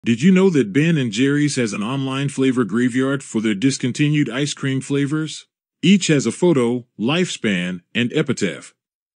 Do you know about this amazing fact about Ben and Jerry's ice cream?
Through an online flavour graveyard, Ben & Jerry's pays tribute to their discontinued ice cream flavours. Each flavour receives a digital tombstone with a photo, its lifespan in stores, and a whimsical epitaph. This imaginative tribute allows fans to remember about their favourite sweet delights that are no longer available, bringing a sense of nostalgia to the ice cream experience.